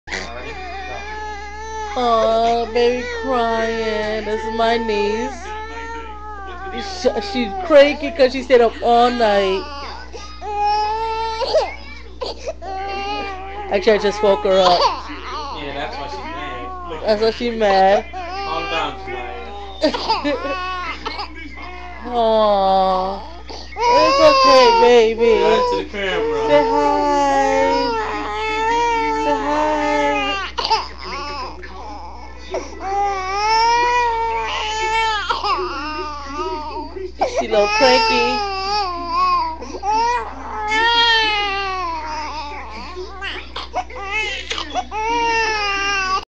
Baby Neice cranky
Mama's was just woken up.